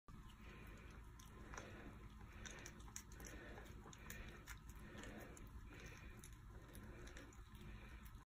Cat bowl, Non-slip Cat Food Bowl, Cat Water Bowl, Cat Feeding Bowl, Small Dog Bowl.
Why choose lesotc Cat Bowl?

Feature:

SAFE FOR PETS: Manufactured with food-grade melamine material. Made from BPA free melamine, non-toxic, no odor, strong and non-friable.

EASY TO USE: These pet bowls for cats are more durable than ceramic cat bowls. They do not make as much noise as stainless steel cat dishes and do not absorb smell like plastic pet bowls. Dishwasher safe. Do not microwave.

STRESS-FREE FEEDING: Patented shallow and Angled allows your cat to get the food easily, which prevents whisker fatigue and keeps his/her face clean.

NON SLIP: Anti-skid rubber at the bottom prevents slipping and spilling, and protects floor from scratches.